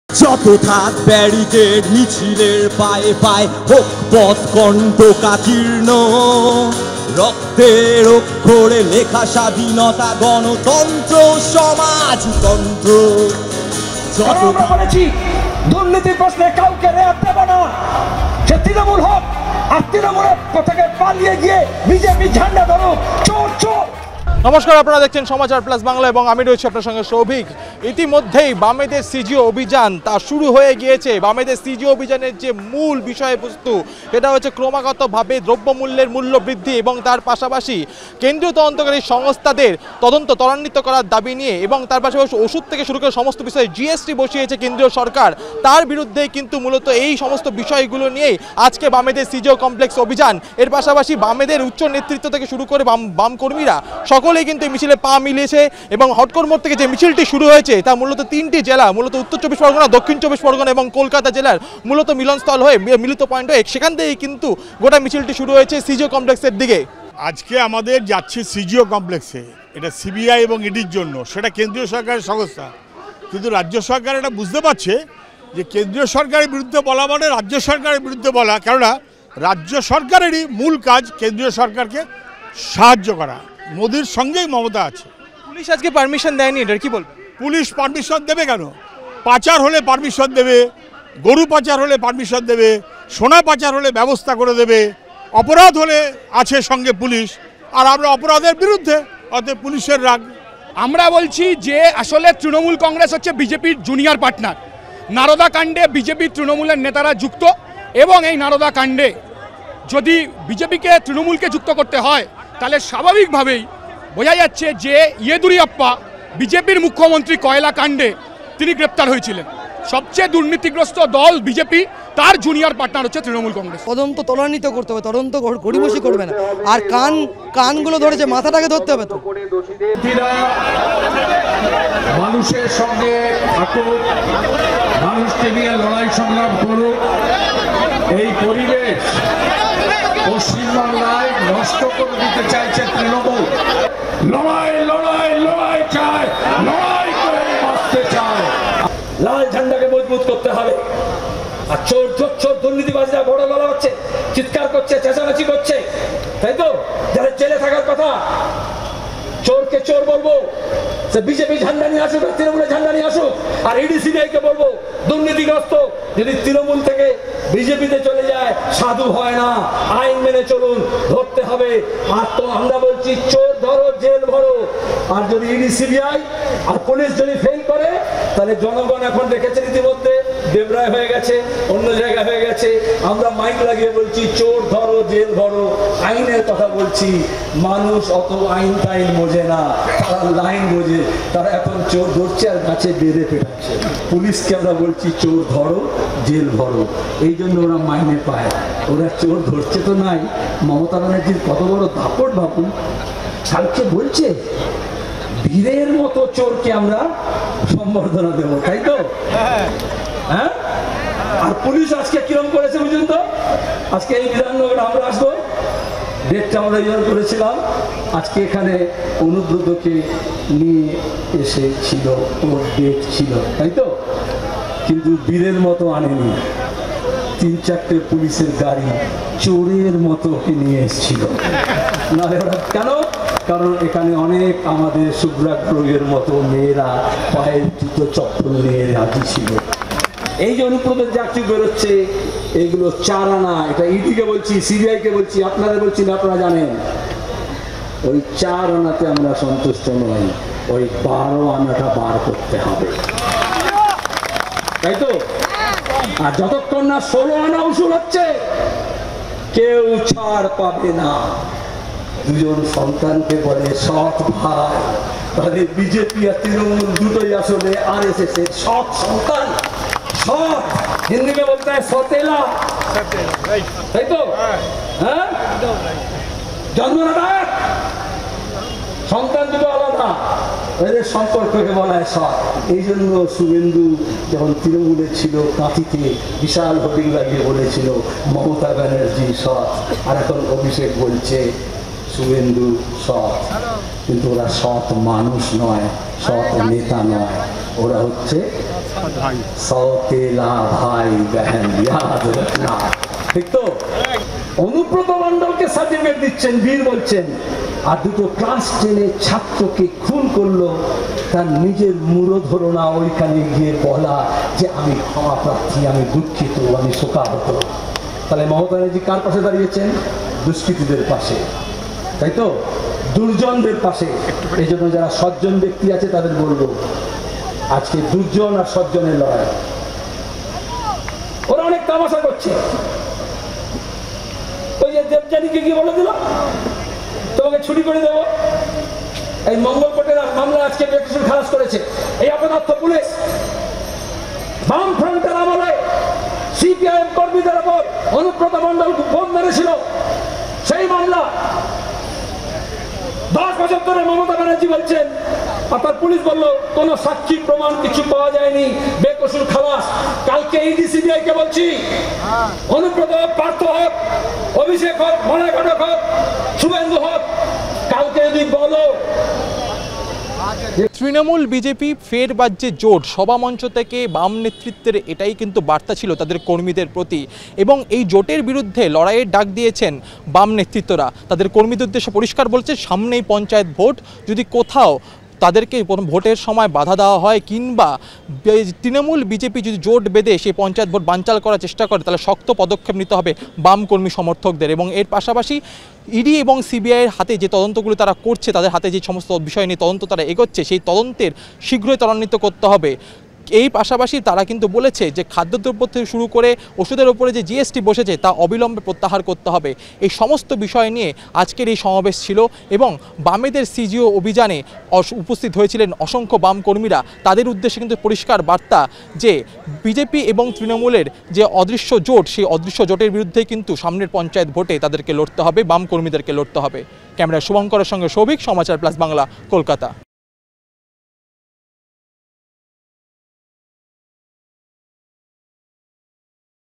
Left Front CGO complex :বামেদের CGO অভিযান, বিস্ফোরক মন্তব্য বাম নেতৃত্বের। কি বললেন সুজন সেলিম ?
Left Front CGO complex Abhijan:বামেদের CGO অভিযান, বিস্ফোরক মন্তব্য বাম নেতৃত্বের। কি বললেন সুজন সেলিম ?